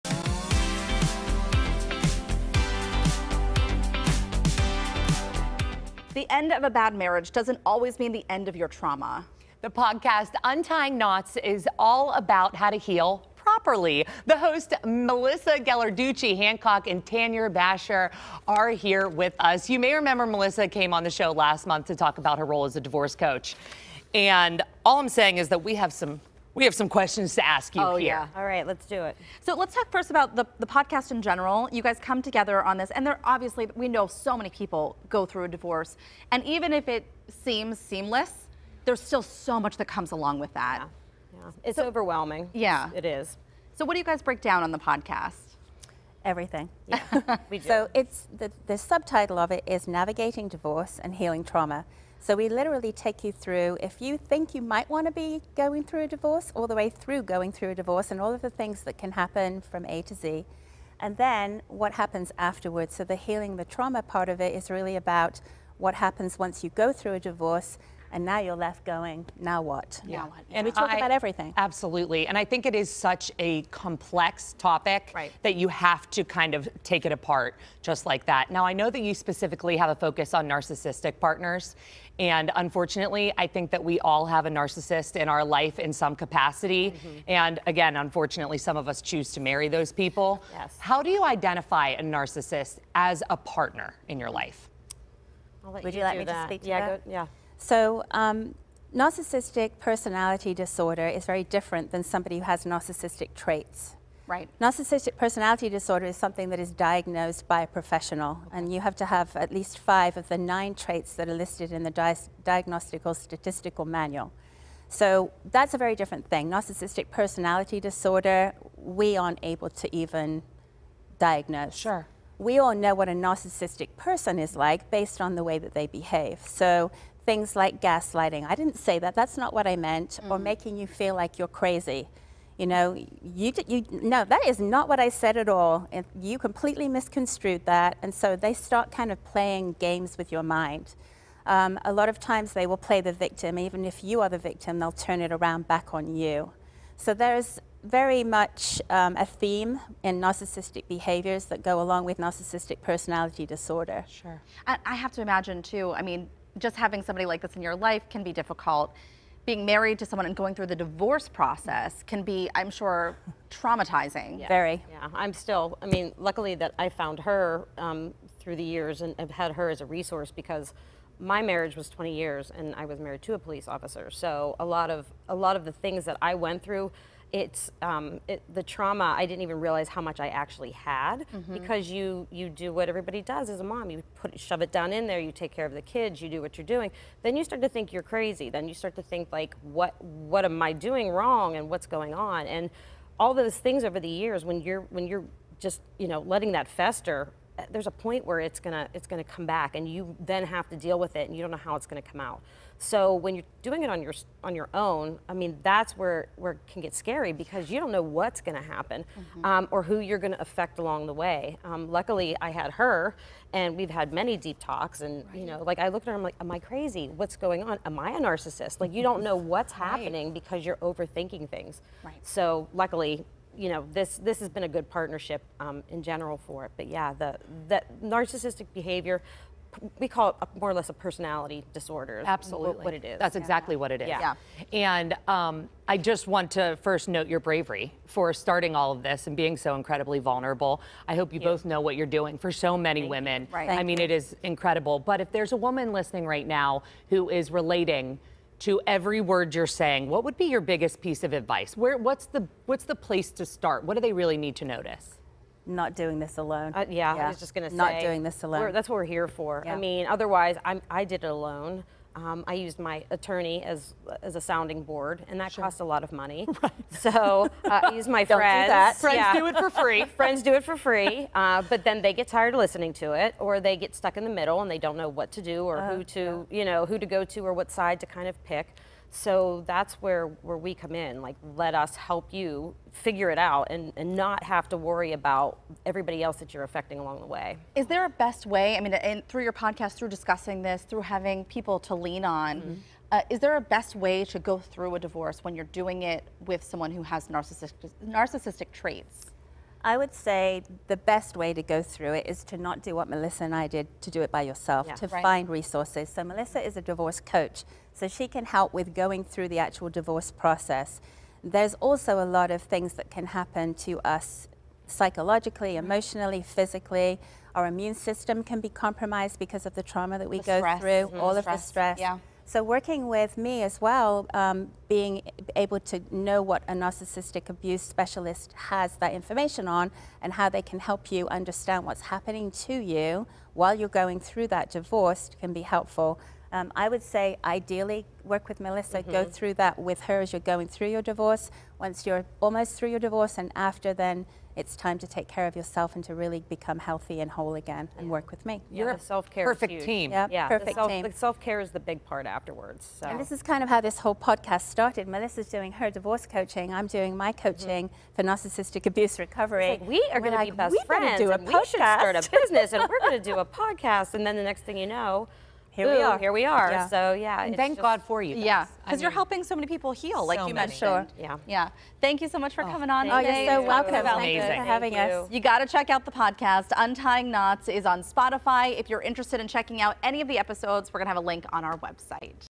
Hosts of the 'Untying Knots Podcast' share how to start the healing process from divorce
The hosts of the "Untying Knots Podcast," Melissa Ghelarducci Hancock and Tanya Bashor, joined us to share more about their podcast and how it can help.